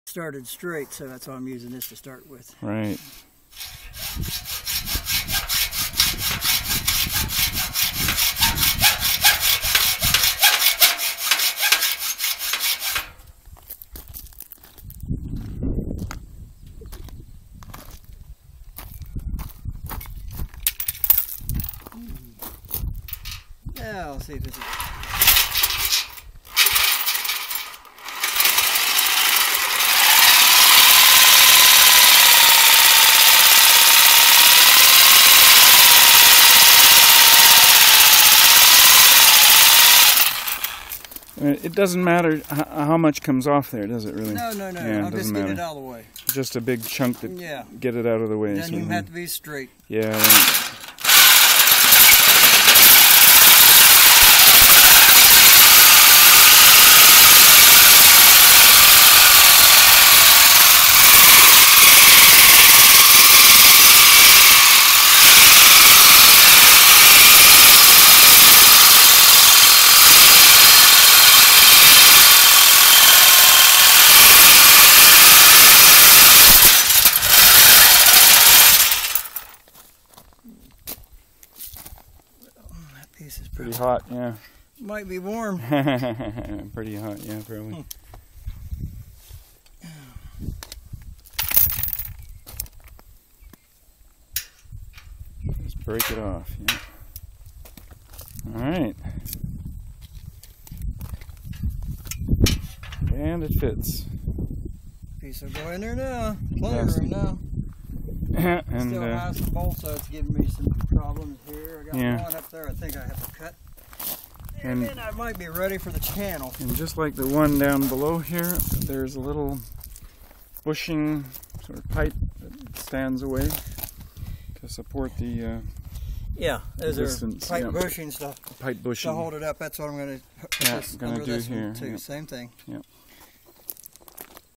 track on truss
In this video, Mike Brown shows how we are making the placement and attachment of the "wiggle wire" track the backbone, which is used to hold and attach the inner film layer. For this rapid prototype we have used angles for web members and found that the ideal placement of the track requires cutting off a corner of the web members.